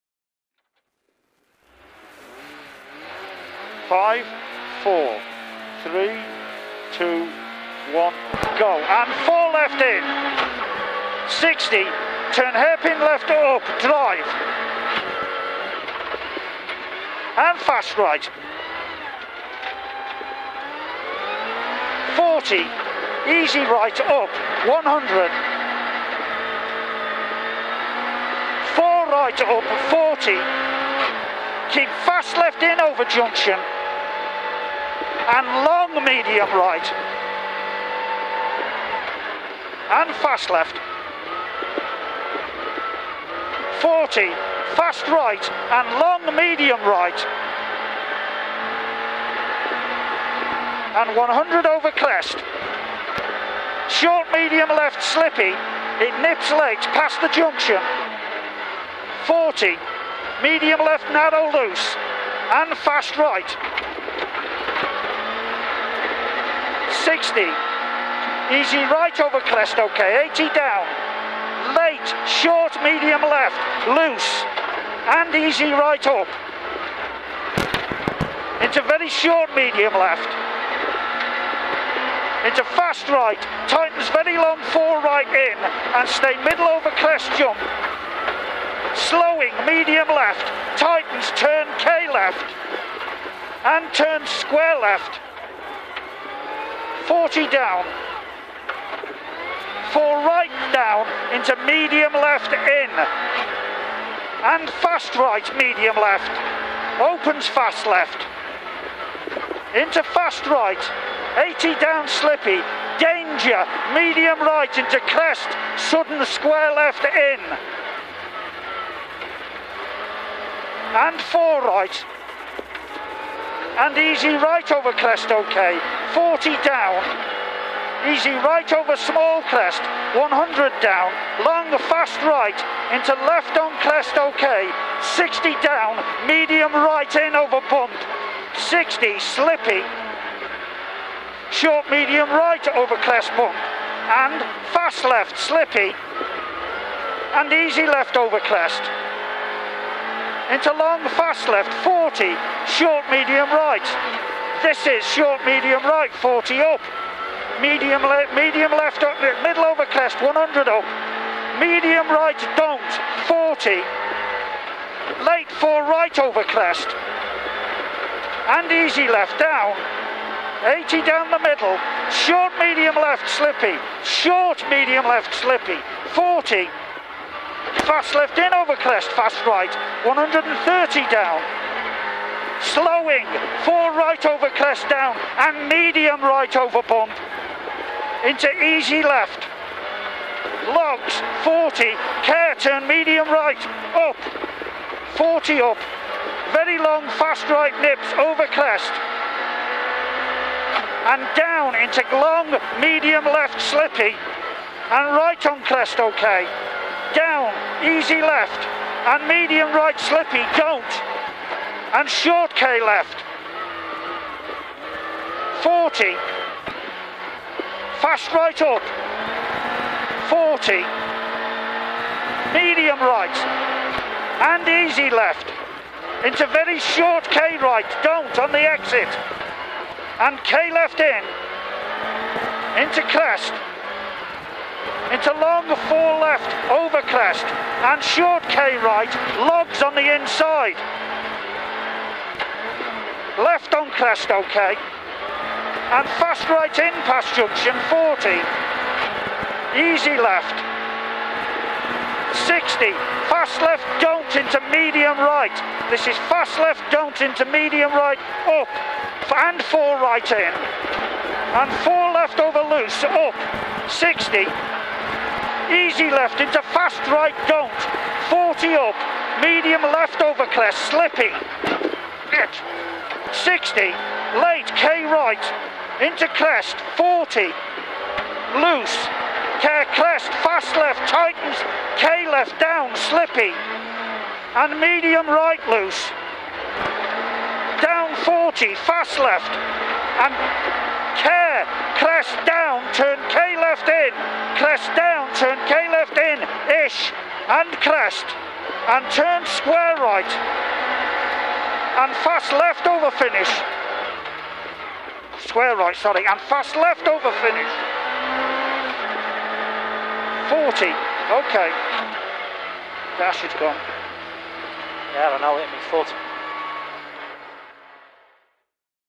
Rally Nuts 2021 - SS3 Hafren south 1 - Tony Simpson & Ian Bevan
MRF Tyres, Service & Sport, SEACON UK, Home 2 Office, Kielder WT